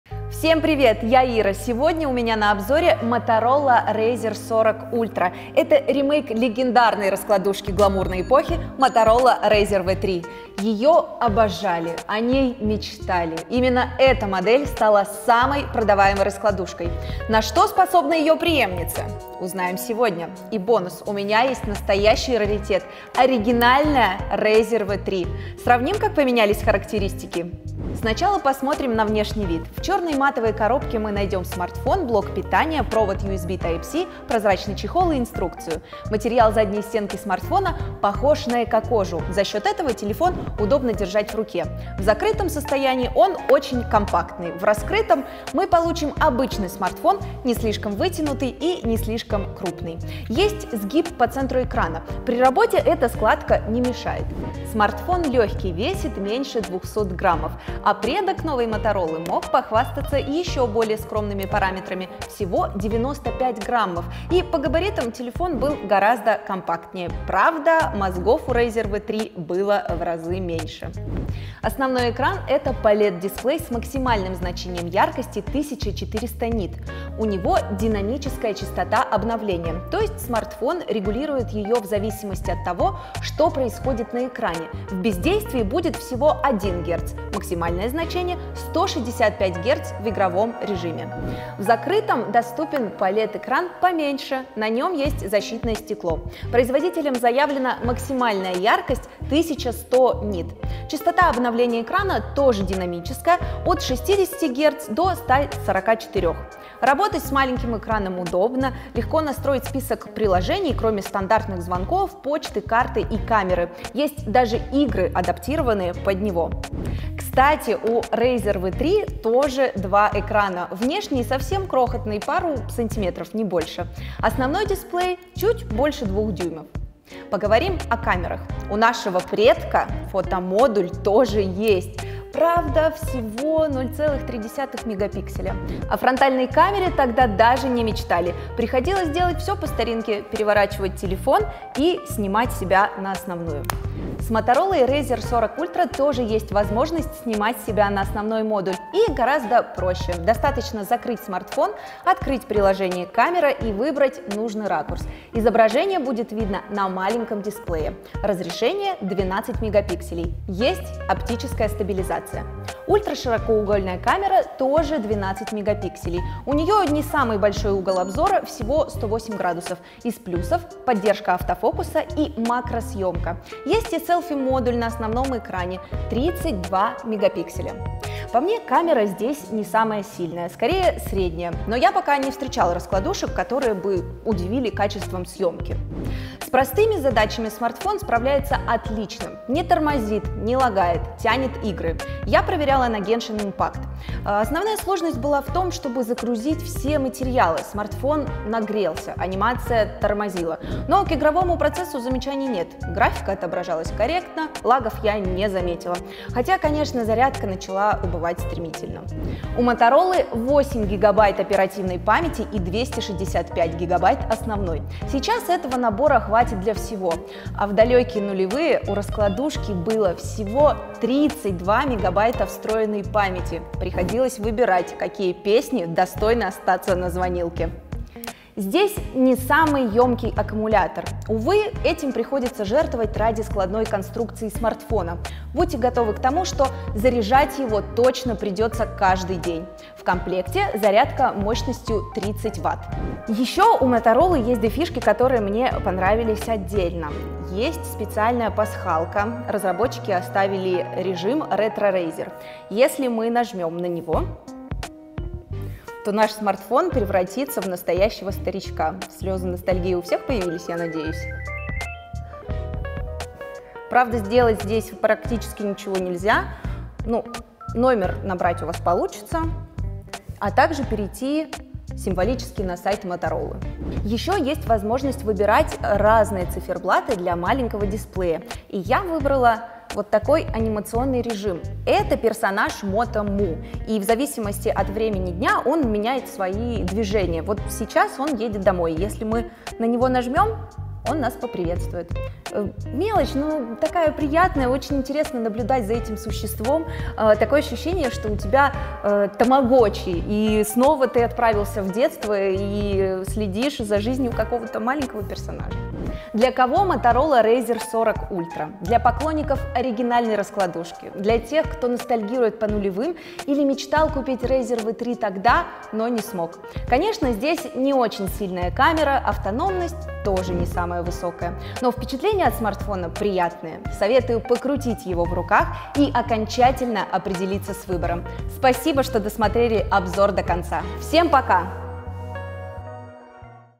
Обзор Motorola Razr 40 ULTRA: ГЛАВНАЯ РАСКЛАДУШКА НУЛЕВЫХ ВЕРНУЛАСЬ?
Смартфон Motorola Razr 40 Ultra — лучший смартфон, если вы ностальгировали по старой модели. Компактный и удобный для ношения, например, в кармане. Основной экран (в развернутом виде),  диагональю 6,9 дюйма и разрешением 1080x2640 пикселей — выполнен из технологии POLED. Плюс для тех, кто хочет высокую яркость, контрастность и реалистичную цветопередачу. Частота обновления 144 Гц обеспечит плавность отображения любого динамичного контента, включая самые мовременные игры. А дисплей покрыт стеклом Corning Gorilla Glass 7. Что ещё? Детали в обзоре!

00:00 Знакомство
00:28 Комплектация
00:36 Материалы и сборка смартфона
00:50 Сгиб экрана
00:56 Характеристики и габариты
01:11 Основной экран
01:31 Как работает в сложенном виде?
01:57 Отсылка к старой версии
02:24 Протестировали камеры
03:06 Играем в игры
03:29 Сколько тут памяти?
03:50 Аккумулятор
04:03 Дополнительные фишки
05:07 Итоги